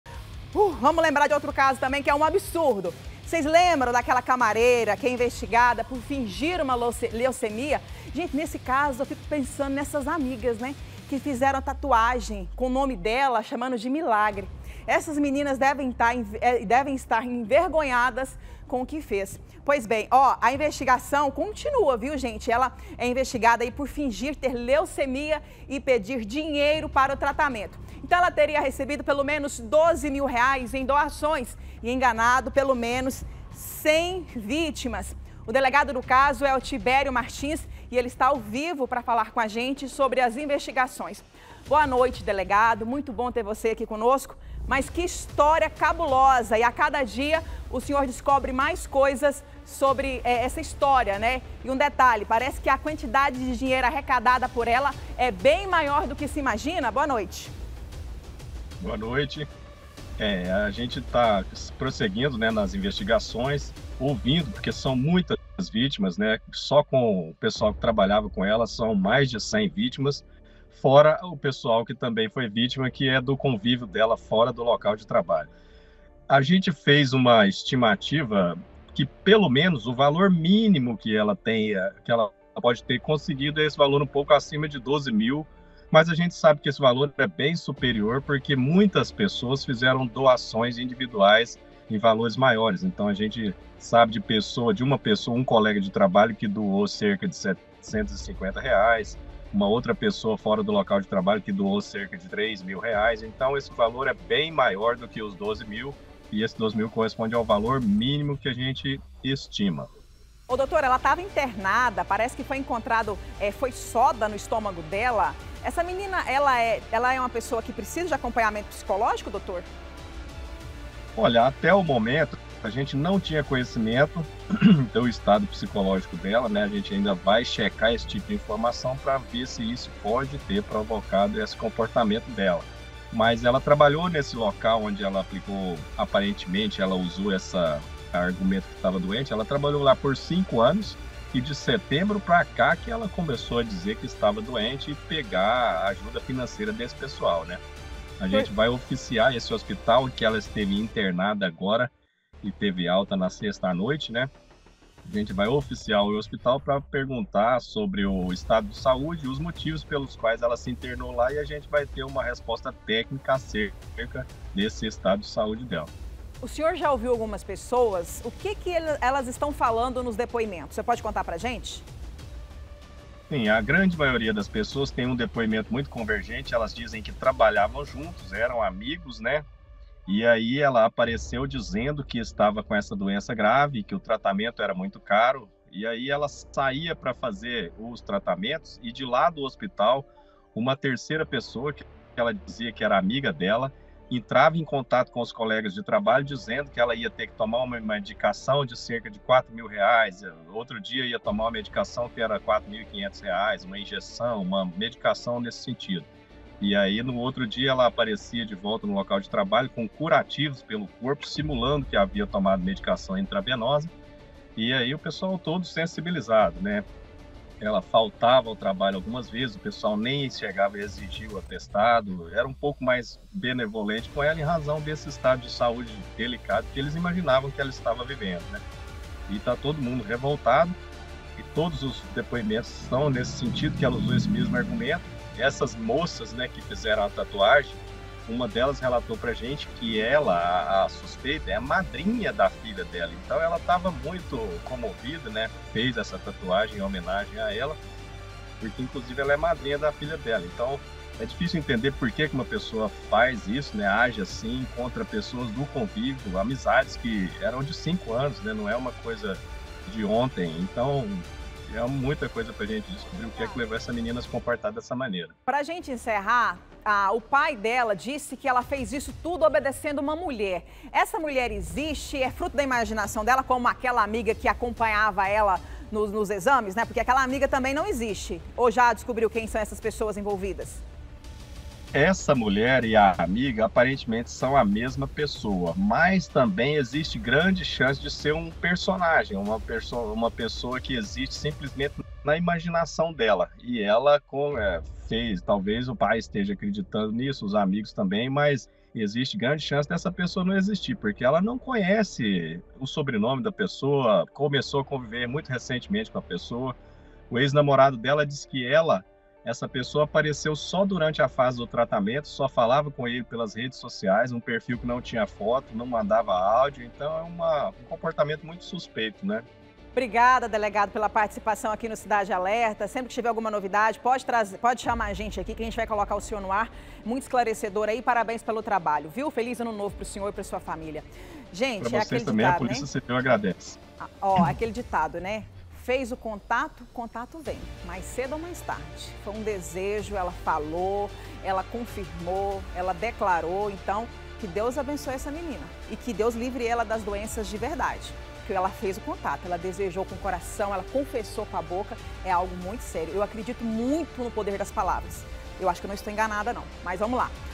GOLPE DA LEUCEMIA: SUSPEITA DE FINGIR DOENÇA DEU PREJUÍZO DE R$12 MIL
LEMBRA DAQUELA CAMAREIRA QUE É INVESTIGADA POR FINGIR UMA LEUCEMIA E PEDIR DINHEIRO PARA O TRATAMENTO?! ENTÃO, ELA TERIA RECEBIDO PELO MENOS 12 MIL REAIS EM DOAÇÕES E ENGANADO PELO MENOS 100 VÍTIMAS. O DELEGADO TIBÉRIO MARTINS ESTÁ AO VIVO PRA FALAR DAS INVESTIGAÇÕES.